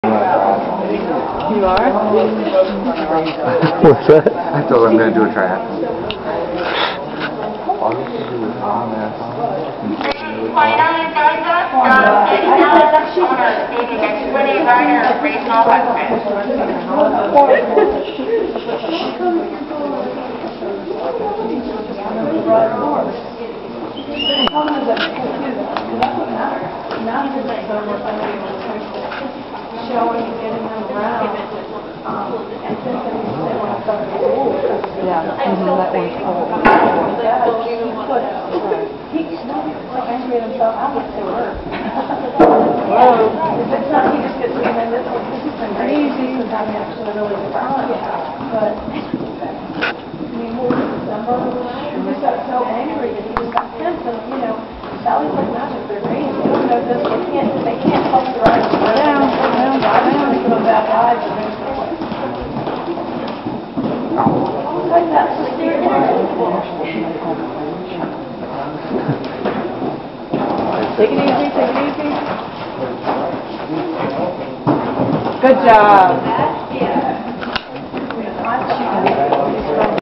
Gone Kaydee Gone, Highpoint Jumper Show, 2/21, 3
Timed First Round, No Jump-Off, Class 3, Puddle Jumpers, 1st Place, Champion for the Division!!